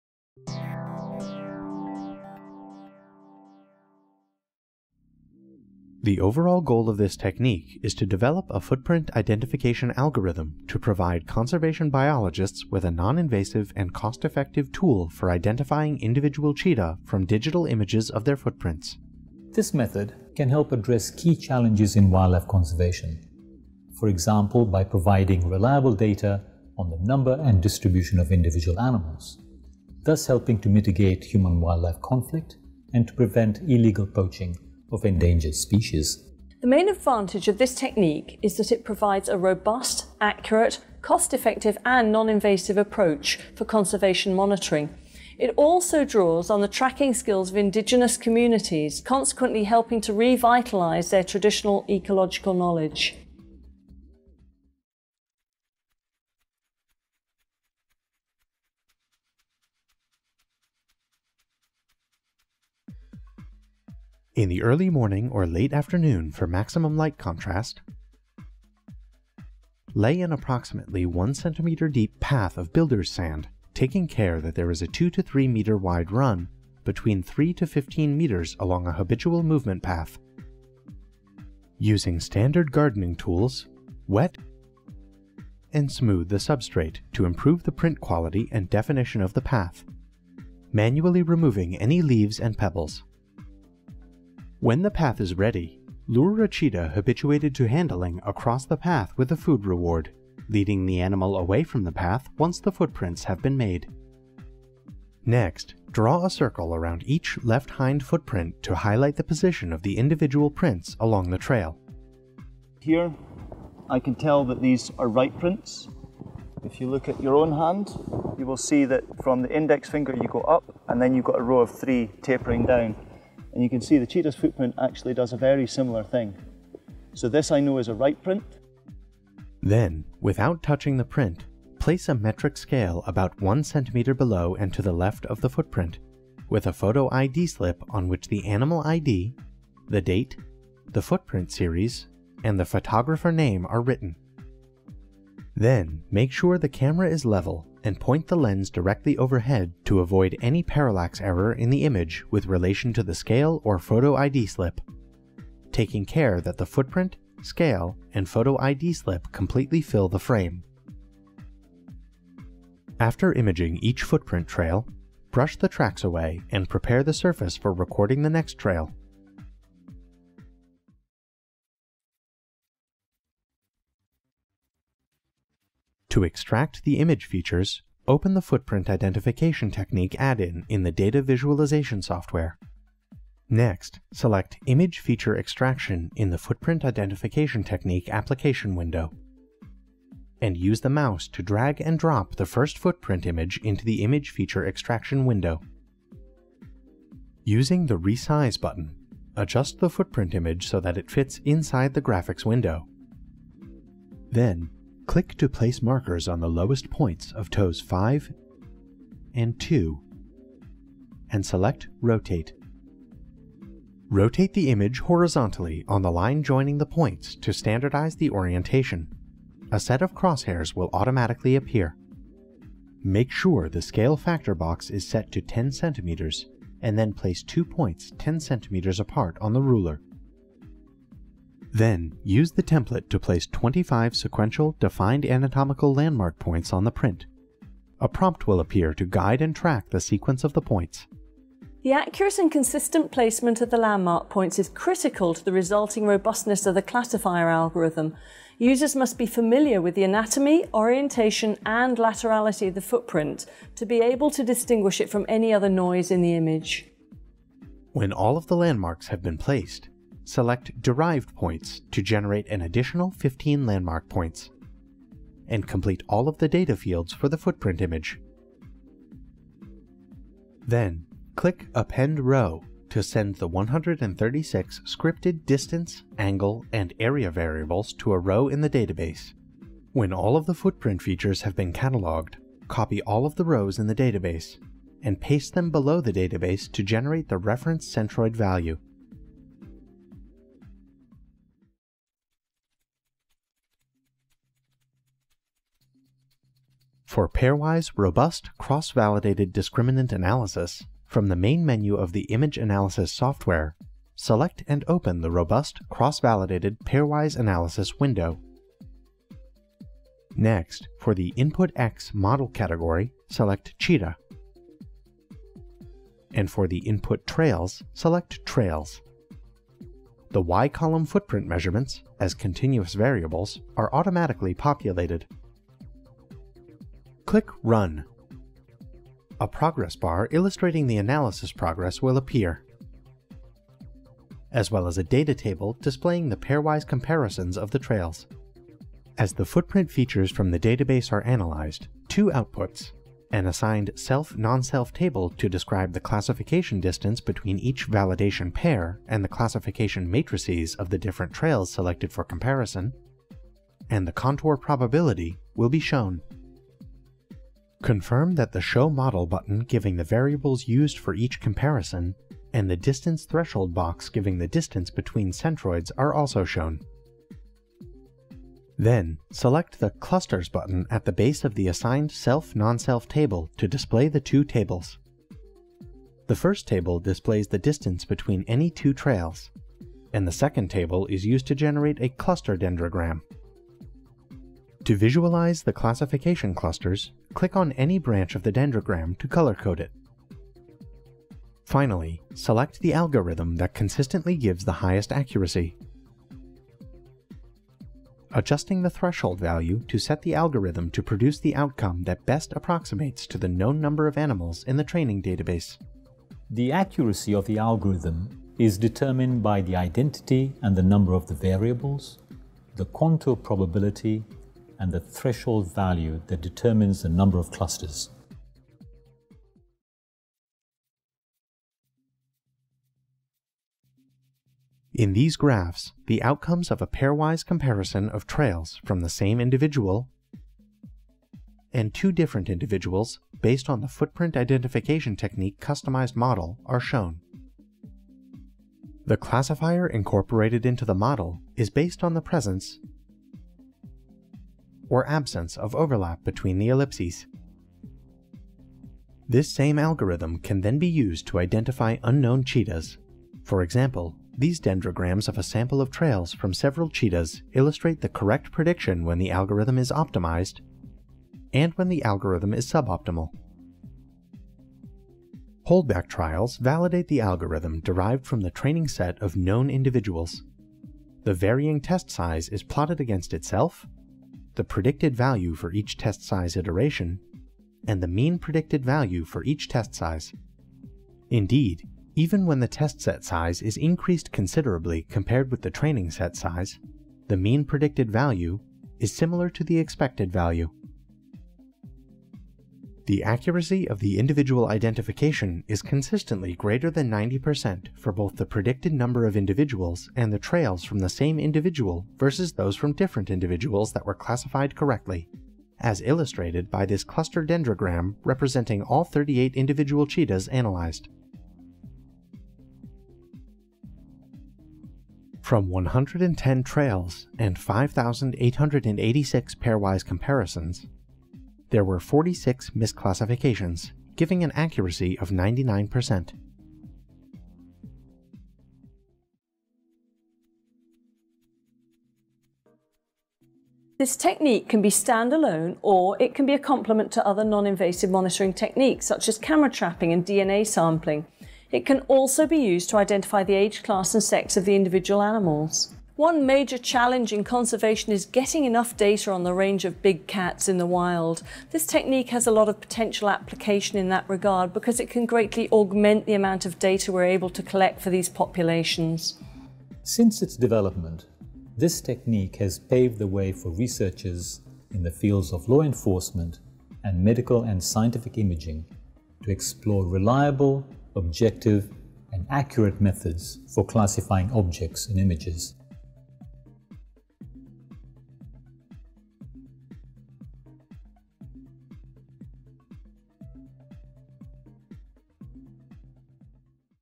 Spotting Cheetahs: Identifying Individuals by Their Footprints
An Open Access Protocol from JoVE Environment.

The cheetah (Acinonyx jubatus) is an iconic, endangered species, but conservation efforts are challenged by habitat shrinkage and conflict with commercial farmers. The footprint identification technique, a robust, accurate and cost-effective image classification system, is a new approach to monitoring cheetahs.